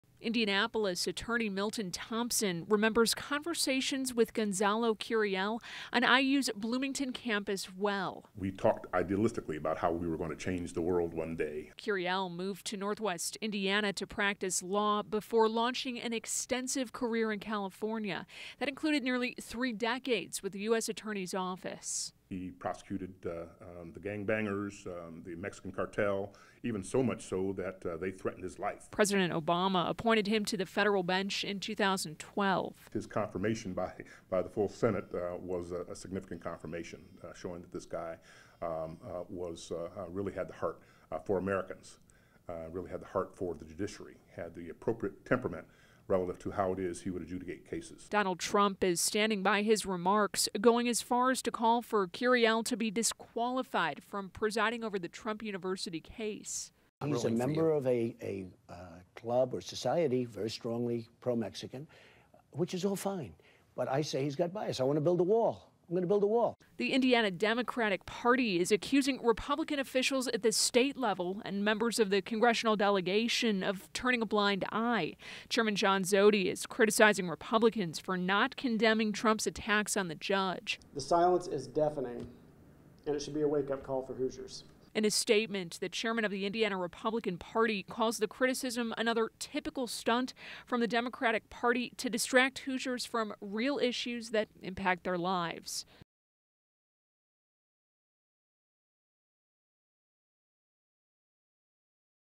Law school friend defends Indiana-born judge under attack by Donald Trump
Milton Thompson says Judge Gonzalo Curiel is a "soldier" fighting for the right thing.